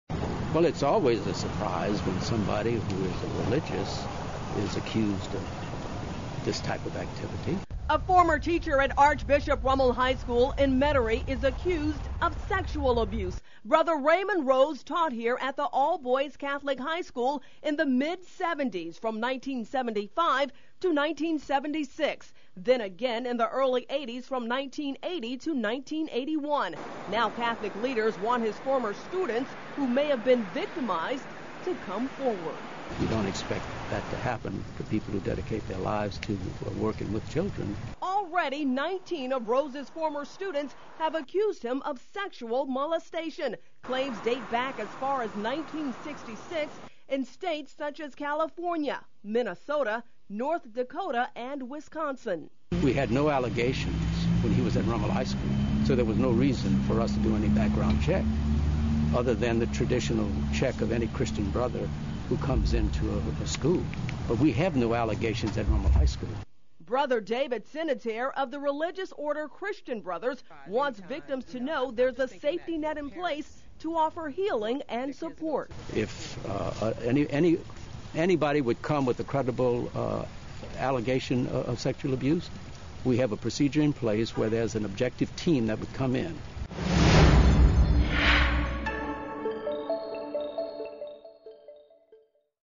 Previous catholic high school teacher charged with sex abuse
Brother Raimond Rose, a former catholic school teacher in Archbishop Rummel High School is charged for sex abuse by his nineteen students that took place in 1966. the authority investigated the allegations but has never charged for the crimes.